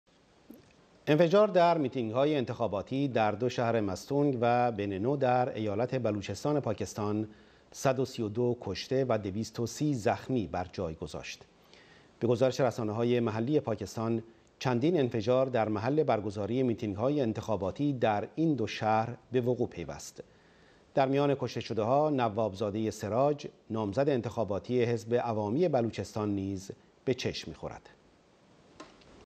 پاکستان کشته و مجروح شدن در انفجارها در متینگ های انتخاباتی
سیمای آزادی تلویزیون ملی ایران ۲۳ تیر ماه ۹۷